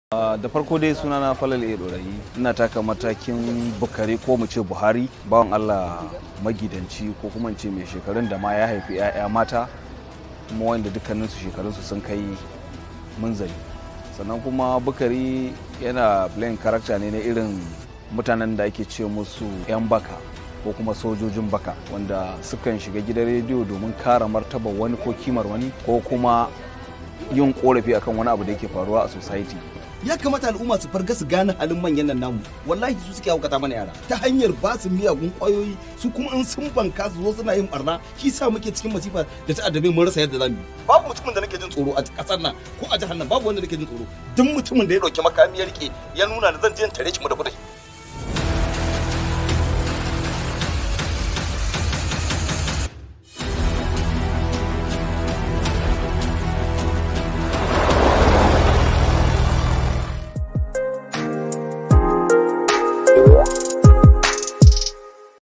Bana Bakwai behind the scenes with Falalu A Dorayi
BANA BAKWAI yana nan tafe kan manhajar Northflix ran 31 July 2022 Inn Shaa Allah.
Rashin aikin yi, talauci, da rashin kulawa ya jefa wasu matasa fadawa cikin harkoki marasa kyau kala-kala wanda ya jawo barna da rashin zaman lafiya a cikin gari. Ana tsaka da haka sai hukumar ‘yan sanda ta nada ASP Nazir domin nazartar lamarin da kuma kawo zaman lafiya da gurfanar da wadannan matasa domin yi musu hukunci.
Yan wasa :
Produced by @realabmaishadda & @nazifiasnanic
Story and Directed by @realalinuhu
A digital platform that hubs HausaMovies for viewership globally. Download our application on Google PlayStore / Apple Store to watch latest Hausa movies.